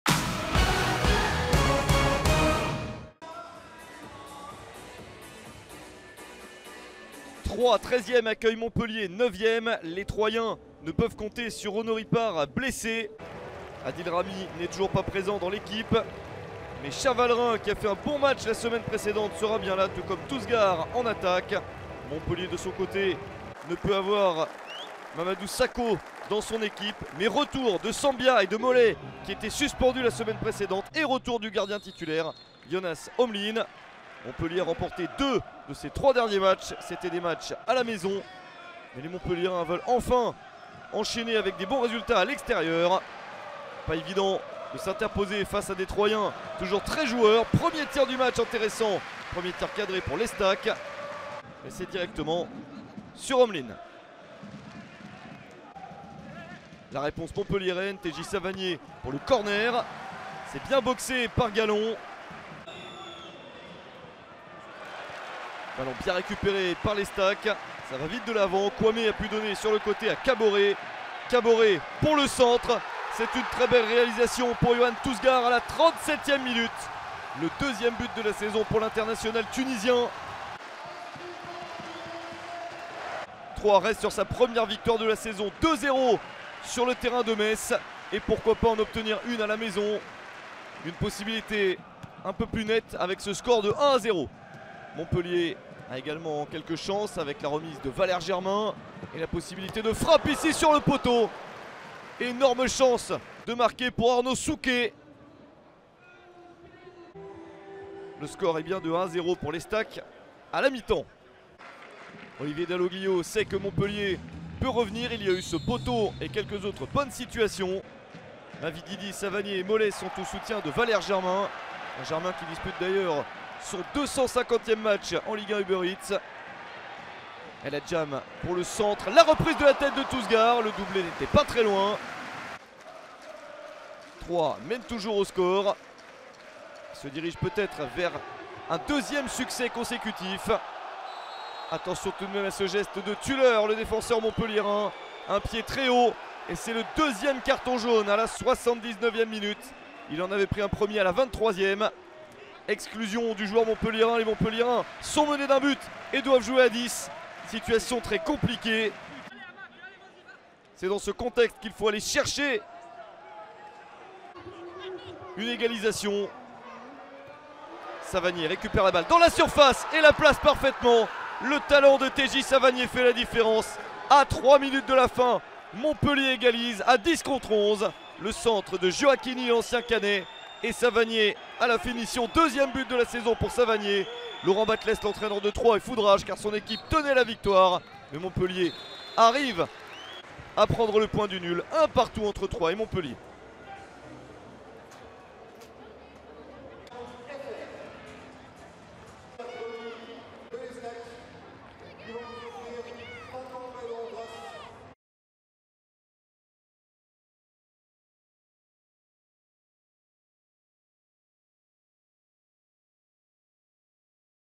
ESTAC TROYES - MONTPELLIER HÉRAULT SC (1 - 1) - Résumé - (ESTAC - MHSC) / 2021-2022
Revivez les meilleurs moments de ESTAC TROYES - MONTPELLIER HÉRAULT SC (1-1) en vidéo.

Ligue 1 Uber Eats - Saison 2021/2022 - 6ème journée STADE DE L' AUBE - dimanche 19 septembre 2021

Buteurs : Yoann TOUZGHAR (37' - ESTAC) / Téji SAVANIER (87' - MHSC)

Cartons rouges/exclusions : Matheus SOARES THULER (80')

Composition ESTAC TROYES : 5 - Tristan DINGOME /24 - Xavier CHAVALERIN /8 - Jimmy GIRAUDON (c) /15 - Karim AZAMOUM /7 - Yoann TOUZGHAR /30 - Gauthier GALLON /10 - Florian TARDIEU /19 - Oualid EL HAJJAM /4 - Giulian BIANCONE /6 - Rominigue KOUAME /29 - Issa KABORÉ

Composition MONTPELLIER HÉRAULT SC : 5 - Tristan DINGOME /24 - Xavier CHAVALERIN /8 - Jimmy GIRAUDON (c) /15 - Karim AZAMOUM /7 - Yoann TOUZGHAR /30 - Gauthier GALLON /10 - Florian TARDIEU /19 - Oualid EL HAJJAM /4 - Giulian BIANCONE /6 - Rominigue KOUAME /29 - Issa KABORÉ